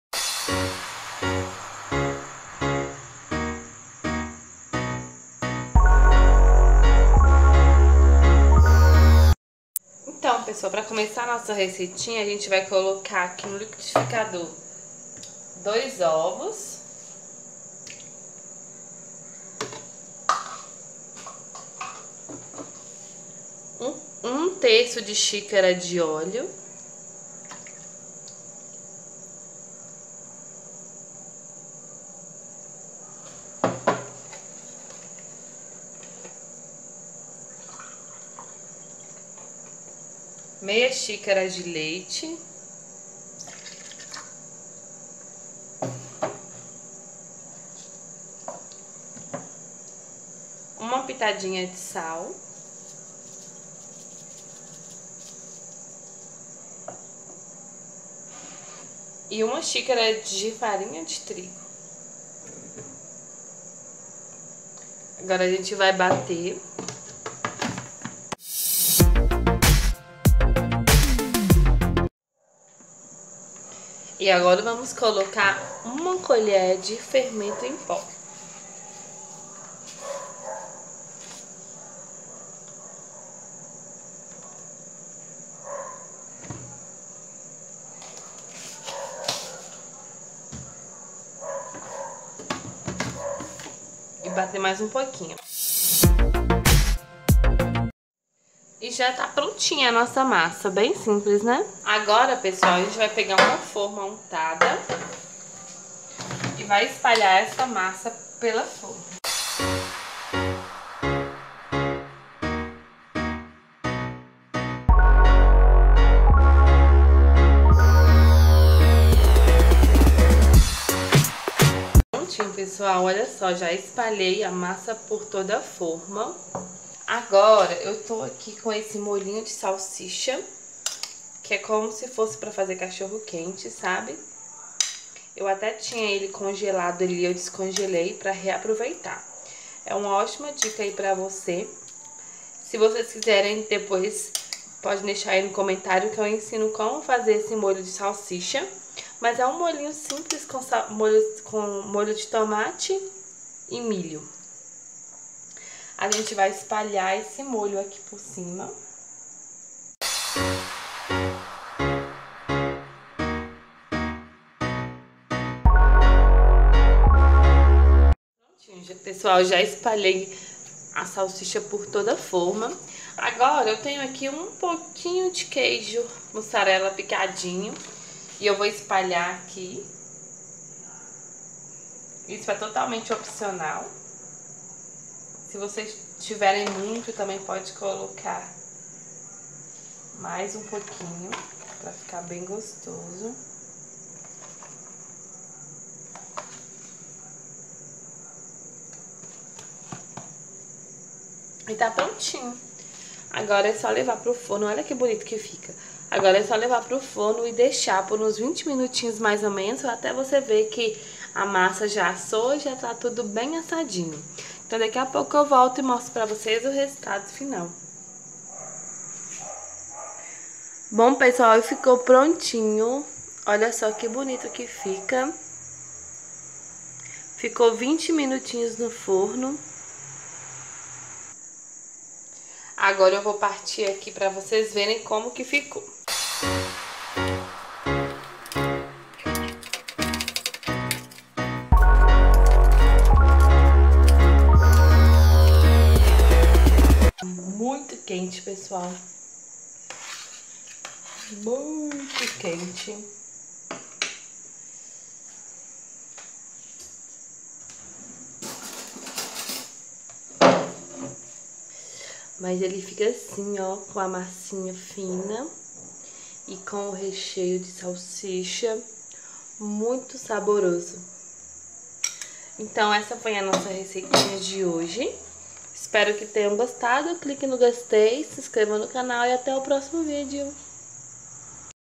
Lanche rápido de salsicha
Se vc está precisando de um lanche bem gostoso e rápido de fazer esta é a melhor opção!!! Sua família vai adorar!